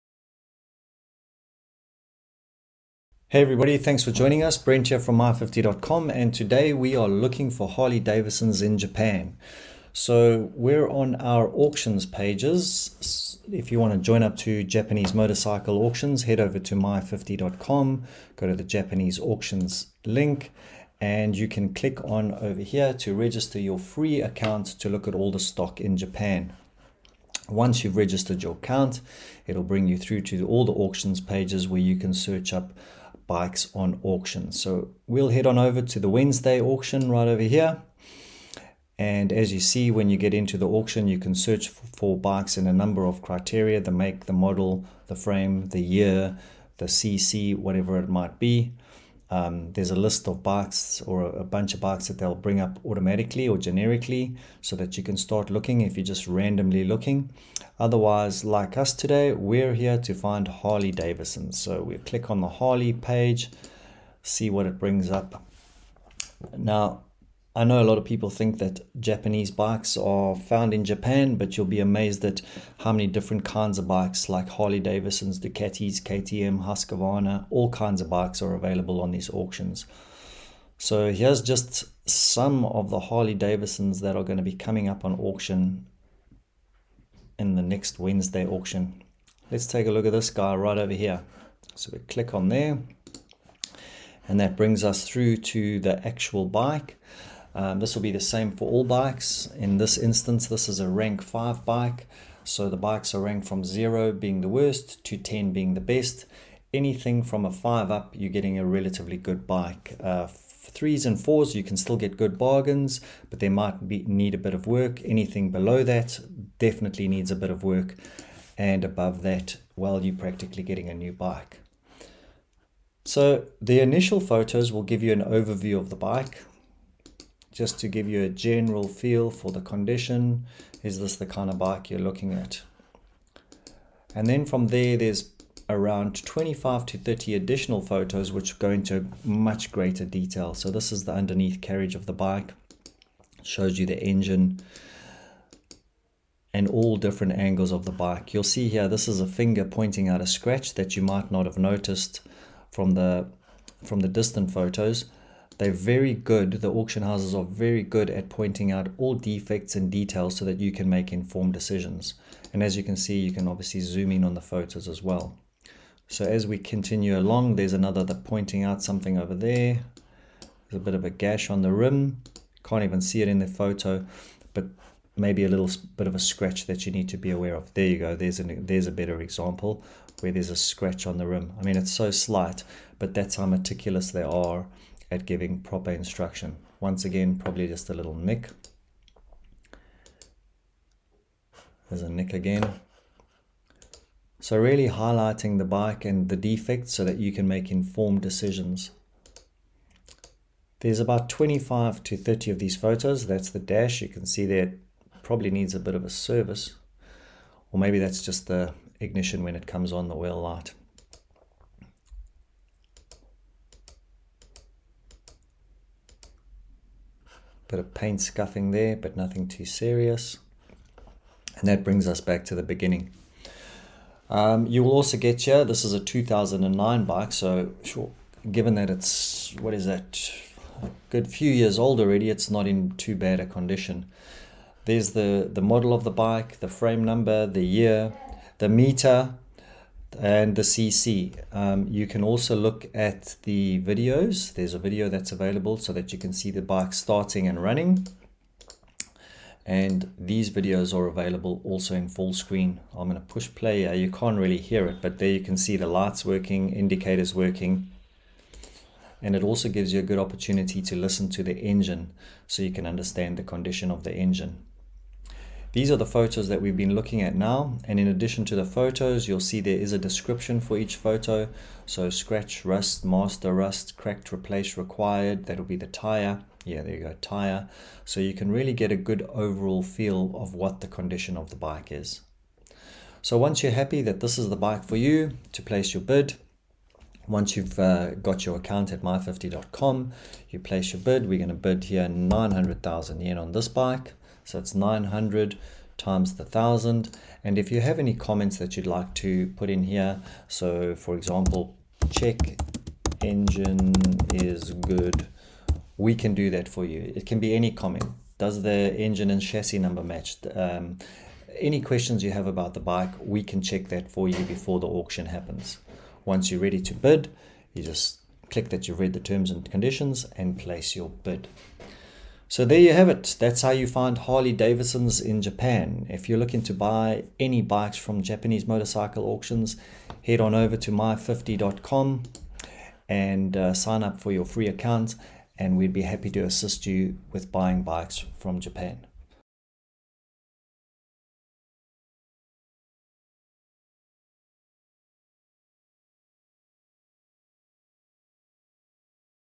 Harley Davidson XL883R I Japanese Motorcycle Actions
Looking to source a Harley Davidson XL883R-I directly from Japan? 🏍️ In this video, we’ll show you exactly how to find and bid on the iconic Harley Davidson XL883R-I using Japanese Motorcycle Auctions with MY50 Motorcycles. Whether you're a dealership or an individual, MY50 Motorcycles gives you access to a wide range of motorcycles, including all major international brands—not just Japanese bikes!

Learn how to set up a free Japanese Motorcycle Auctions account and get insider tips on navigating the auction process to find the perfect Harley Davidson at unbeatable prices.

🔧 What’s in the video:

How to find and bid on Harley Davidson XL883R-I motorcycles
Pro tips for navigating Japanese Motorcycle Auctions
How to source motorcycles from Japan, easily and affordably